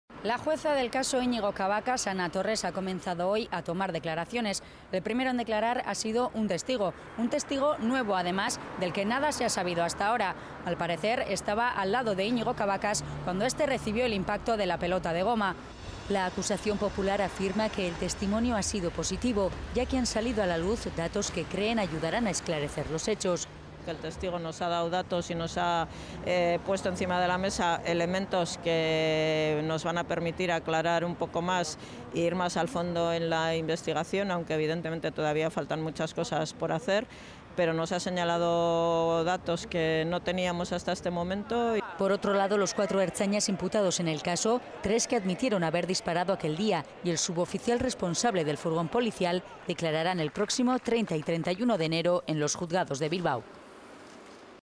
Declara un nuevo testigo en el caso Cabacas
La acusación particular afirma que ha sido un testimonio positivo que ayudará a ir más al fondo en la investigación.